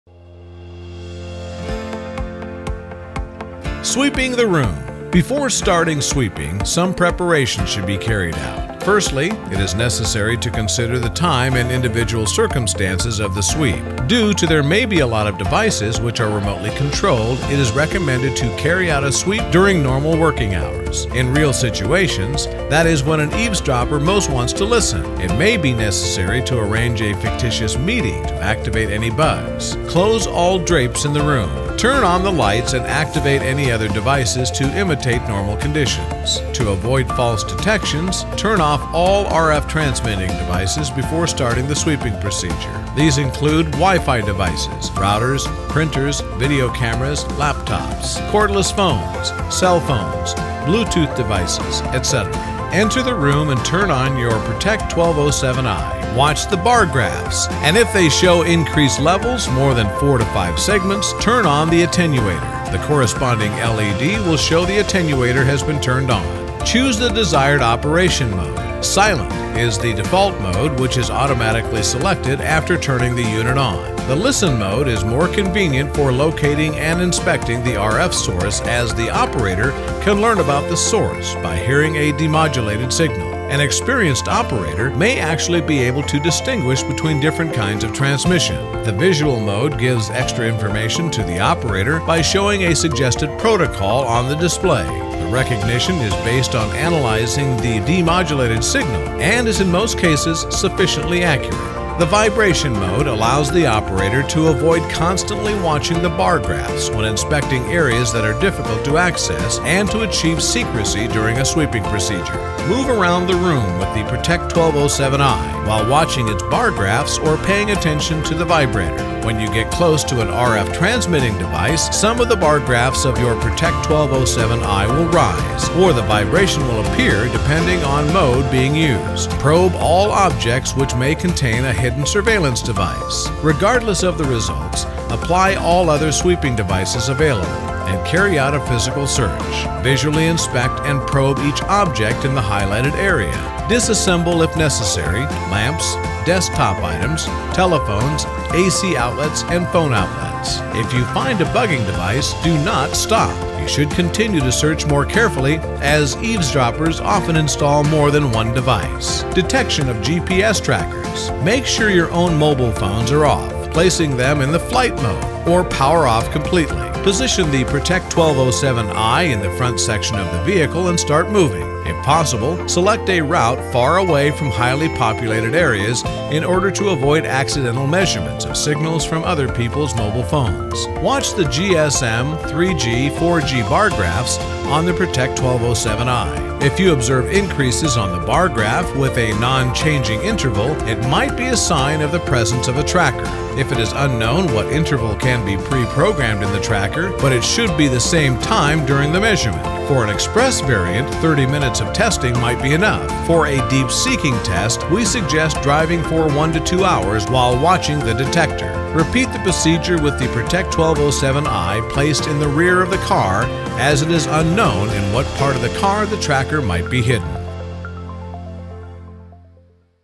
How To Sweep a Room or Vehicle using the iProtect 1207i | KJB Security [DD1207]
When it comes to counter surveillance we often think you need to be an expert, but you don't! This video shows you how easy it is to use the iProtect 1207i [DD1207] Multi-Channel Detector of Wireless Protocols. It's easy to pick up unwanted signals when you are sweeping a room or a vehicle. Follow these steps to ensure you have a successful sweep. The iProtect 1207i detects 6 different frequencies ranges: GSM, CDMA, 3G, 4G, Bluetooth, Wi-Fi & Wi-Max signals.